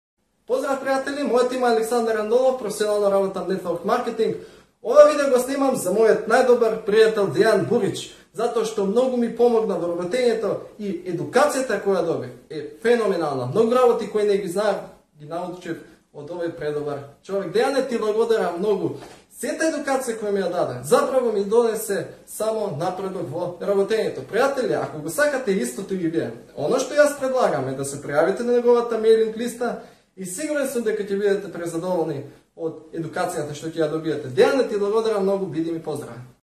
Aleksandar Andonov - Preporuke (Testimonials)
Preporuke ili svedočenja (engl. Testimonials) su veoma bitan deo poslovanja. Traži preporuke od zadovoljnih korisnika tvojih proizvoda ili usluga i one će znatno doprineti tvom brendiranju i sticanju poverenja prema tebi kod budućih potencijalnih kupaca. Hvala Aleksandre :)